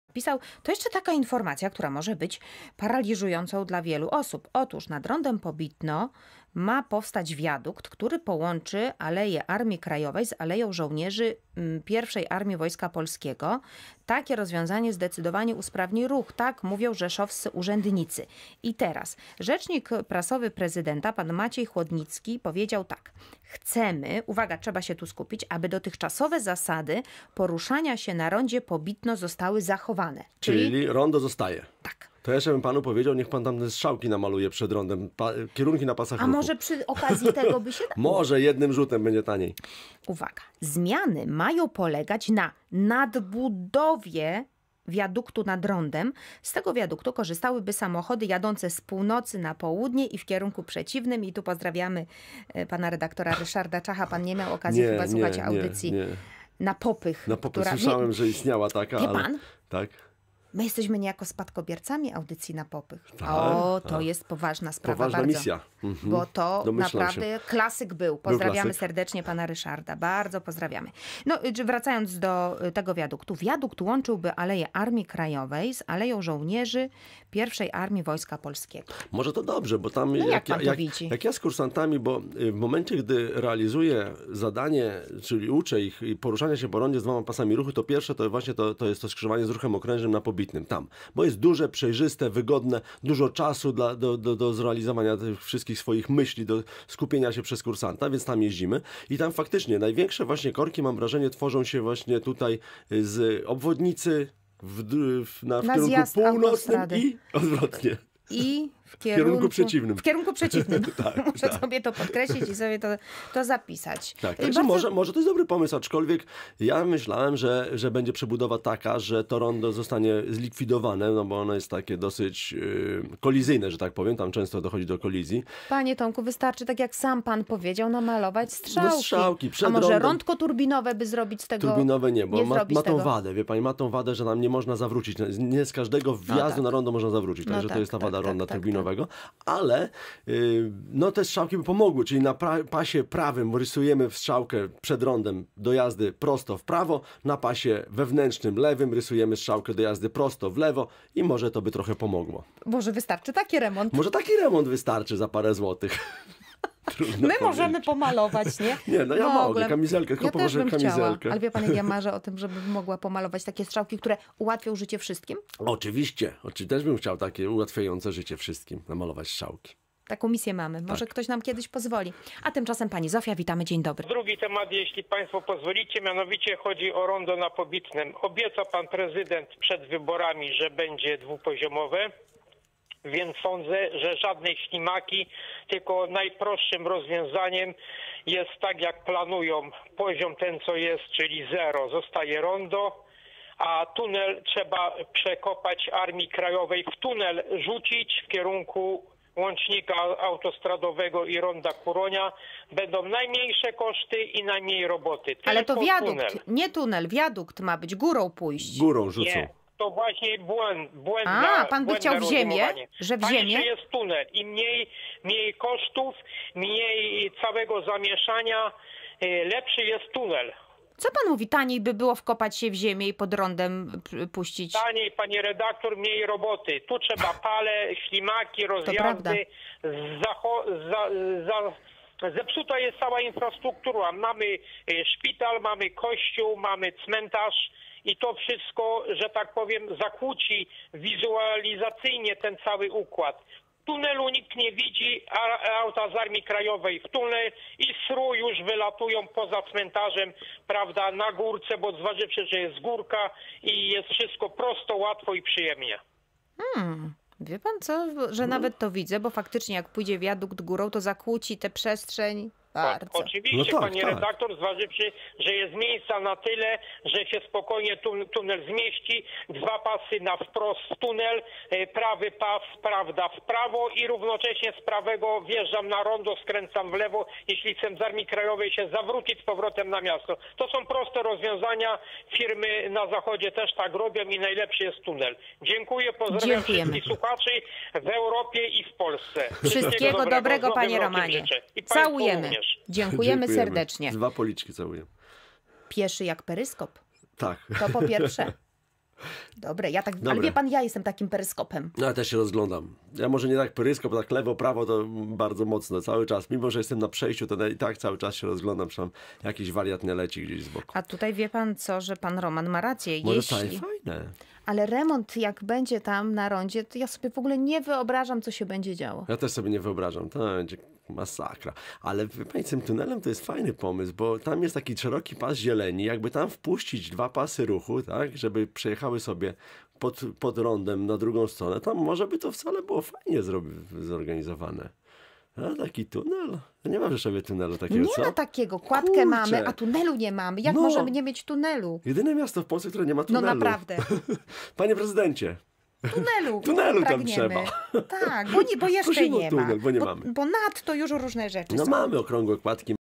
Przebudowa skrzyżowania na Pobitnym
W planach jest przebudowa rzeszowskiego skrzyżowania na Pobitnem. Czy to ma sens?